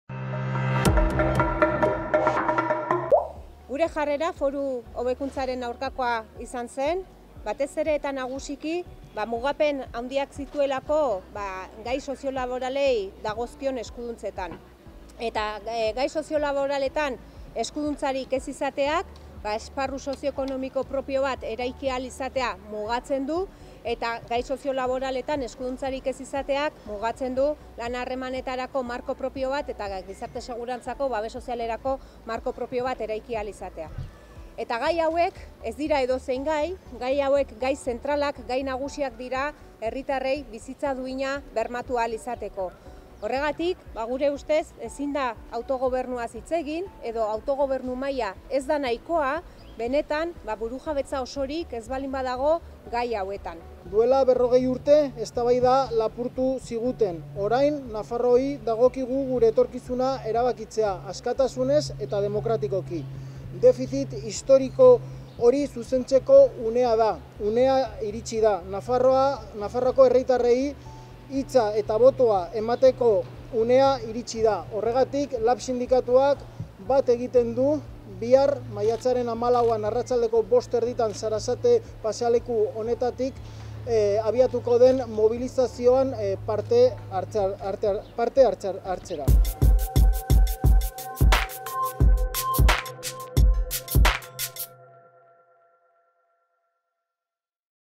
Altxa gaitezen! 40 urteren ondoren, egin dezagun posible!
Garbiñe Aranburu LABeko idazkari nagusiak eta Imanol Karrera Nafarroako bozeramaileak agerraldia egin dute gaur Iruñean, Foruen monumentuaren azpian. Bertan, “Altxa gaitezen! 40 urteren ondoren, egin dezagun posible!” izenburudun adierazpena aurkeztu dute, Foru Hobekuntzaren 40. urteurrenean. Era berean, adierazi dugu LABek bat egiten duela bihar Iruñean, 17:30ean Sarasatetik abiatuko den Navarra XXI Nafarroa-k deitutako manifestazioarekin.